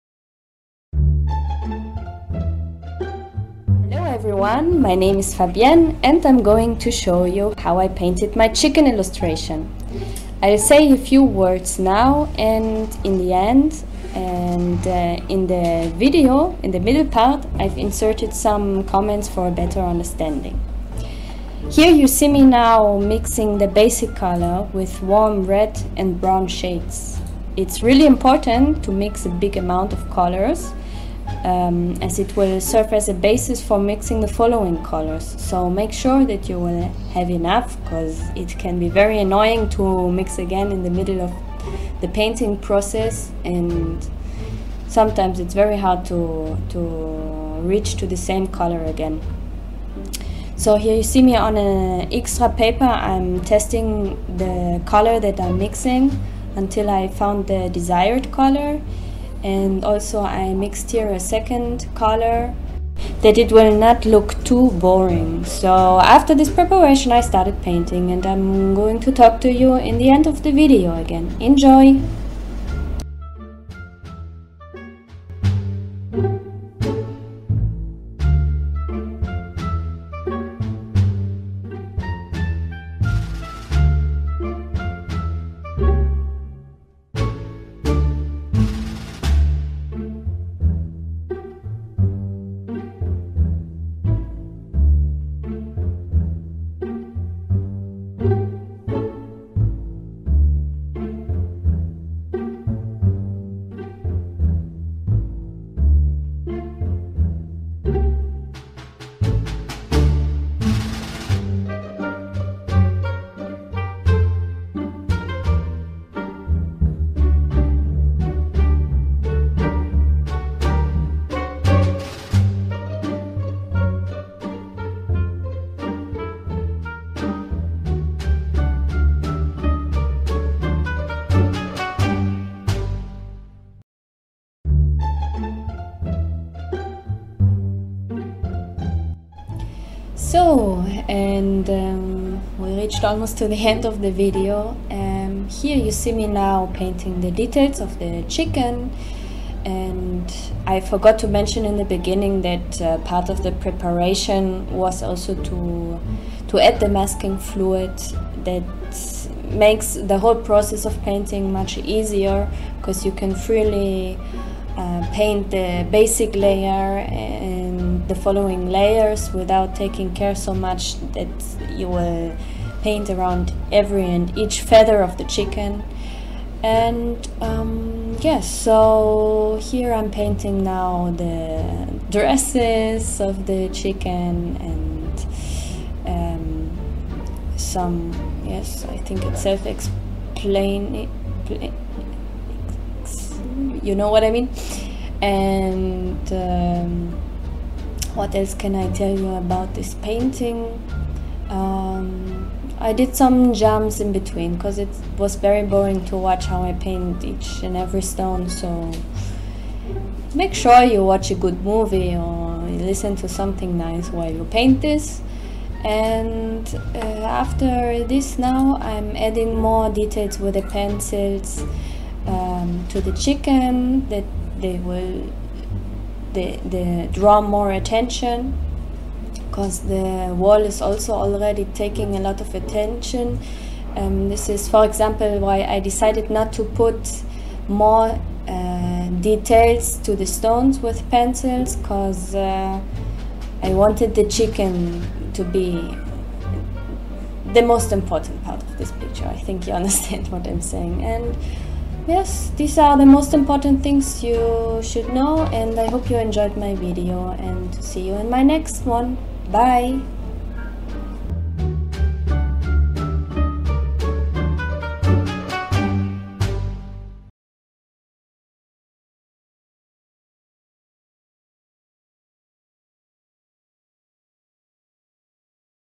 How to paint a chicken ILLUSTRATION in WATERCOLOR
Hello there!
I hope you like my video. If you have any questions, please ask.

Materials I used: Schmincke Horodam and polychromos.